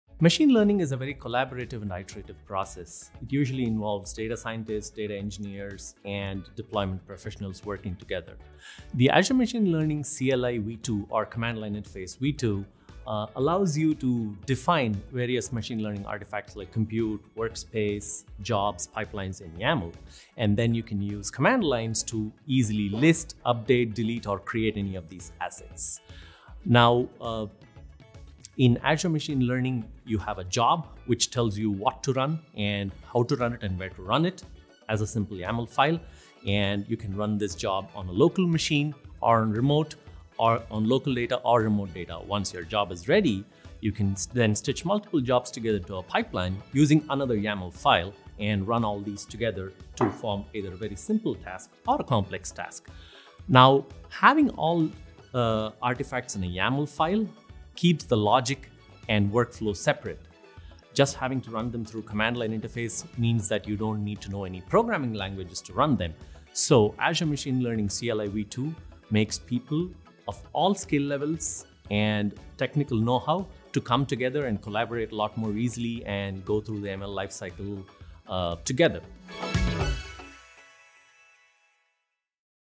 How does Azure Machine Learning CLI v2 enable easy development & collaboration?
Bala Venkataraman explains how the AzureML CLI v2 enables a better collaboration and development environment.